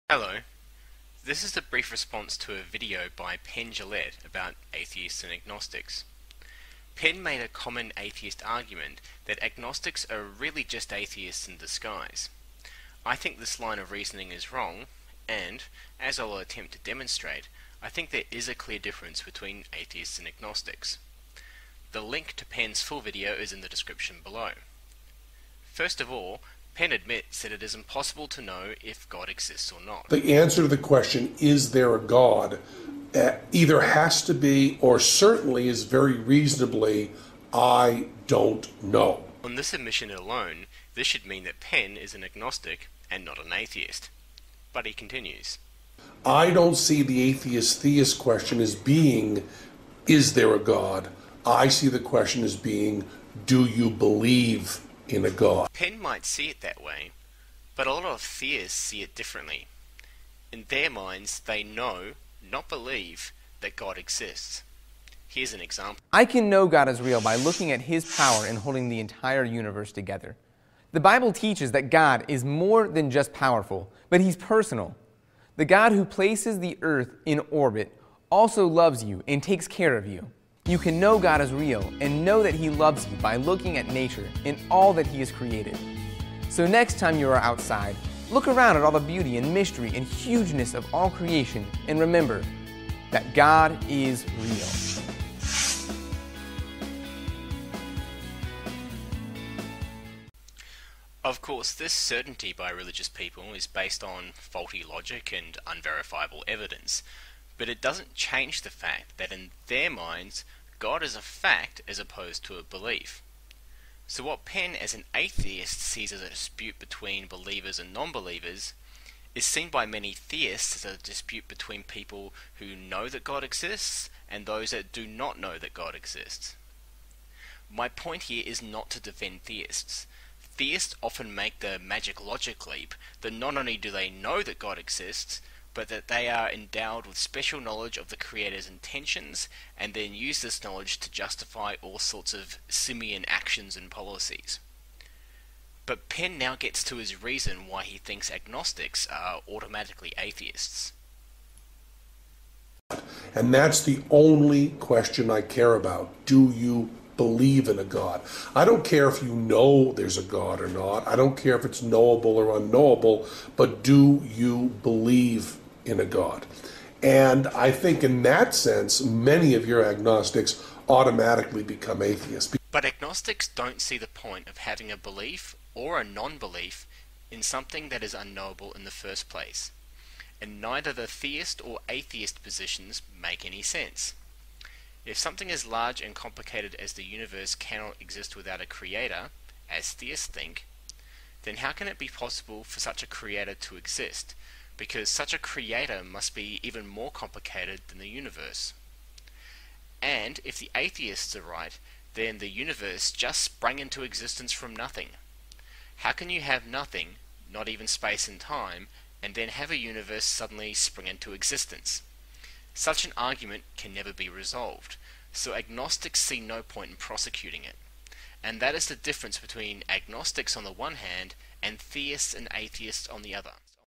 Response to Penn Jillette - Agnostics are not Atheists
And also that religious guys one if you really want to torture yourself for three and a half minutes